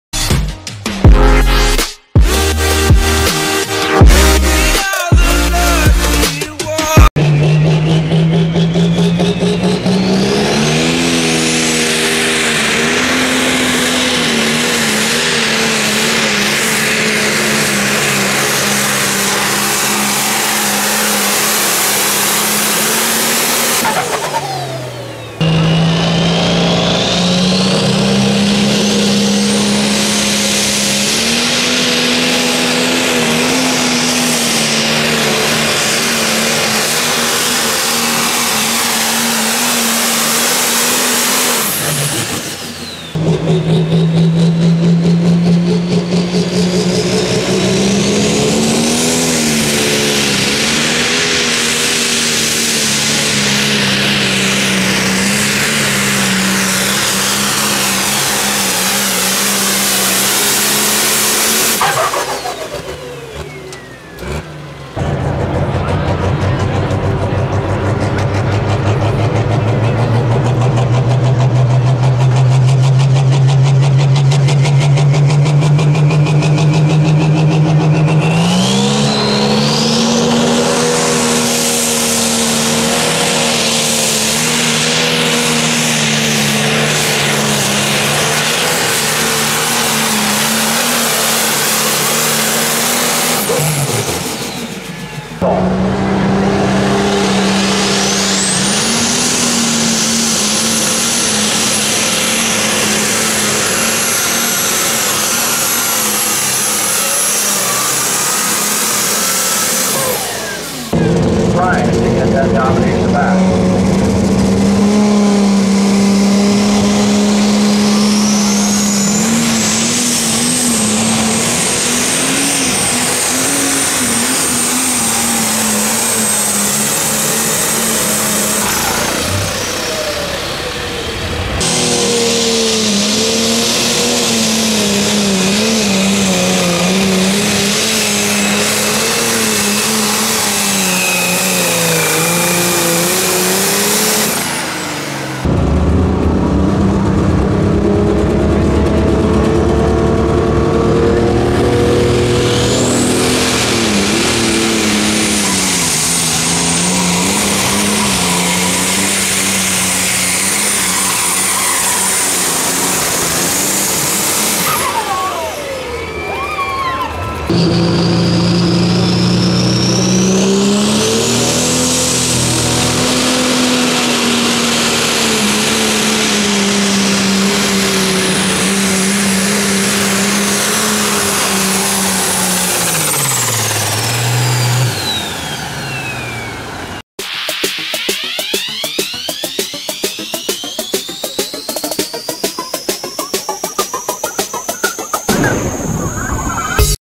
TRACTOR PULL!!!! Super Pro Stock Tractor Pull Lucas Oil East Coast Series
Super Pro Stock Tractor Pull Lucas Oil East Coast Series Buck Motorsports Park 8/10/19